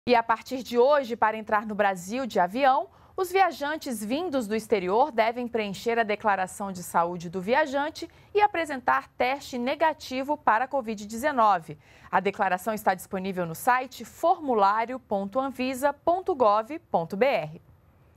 Está em vigor nova regra para viajante entrar no país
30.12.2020 - A partir de hoje, viajantes estrangeiros ou brasileiros vindos do exterior devem apresentar teste negativo de Covid19 e assinar declaração de saúde para entrar no Brasil.